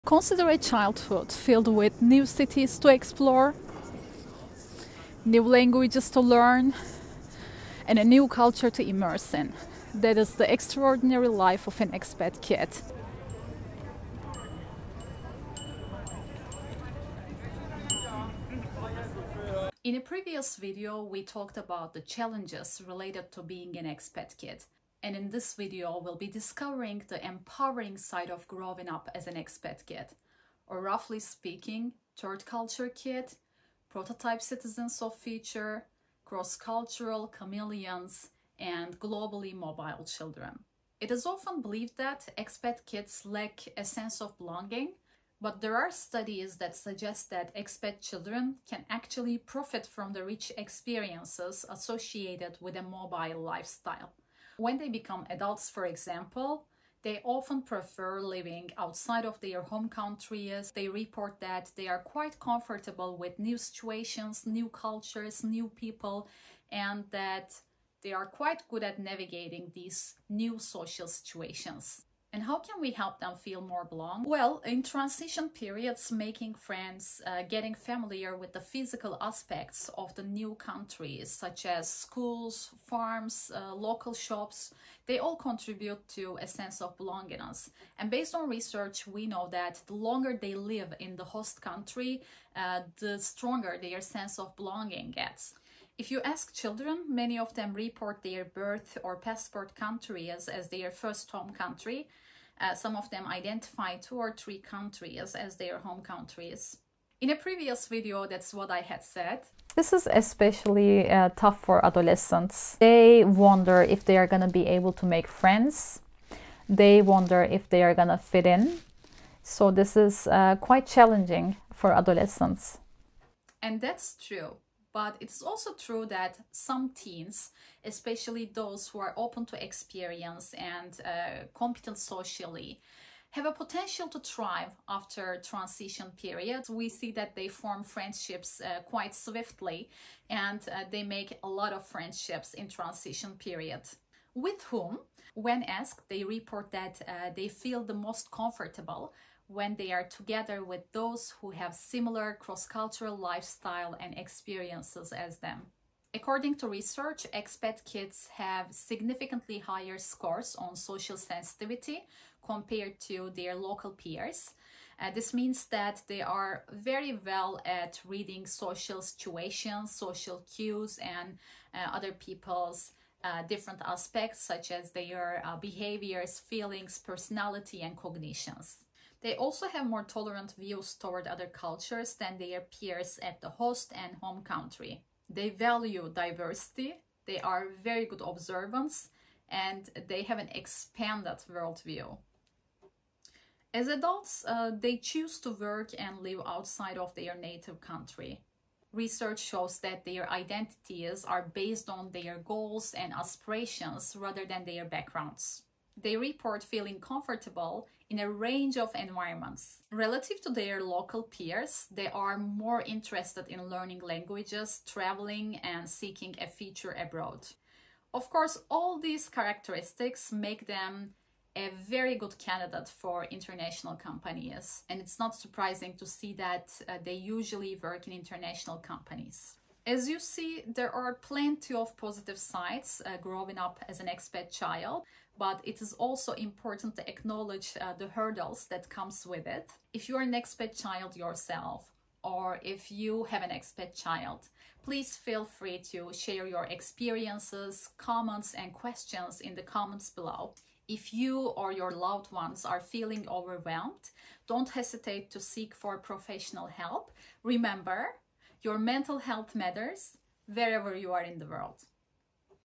☀️ The Bright Side of Expat Childhood | The Empowering Experiences of Third Culture Kids
**💼 Description**

Explore the unique advantages of growing up as an expat child. Join me as I dive into the empowering aspects of living a globally mobile childhood and the impact it has into adulthood.

**🌟 Fun Fact From the Episode 🌟**

Did you know that many expat kids identify not just one but multiple countries as their "home"? That multi-homed feeling really broadens their sense of belonging in the world!

**📚 Timestamped Overview**

00:00 Some teens thrive after transition, form strong friendships.

03:51 Growing up as an expat child: challenges and support.

**👉 Let’s Get the Conversation Going!**

Do you have an expat story to share? Maybe a question or a fascinating experience? Drop a comment on the video.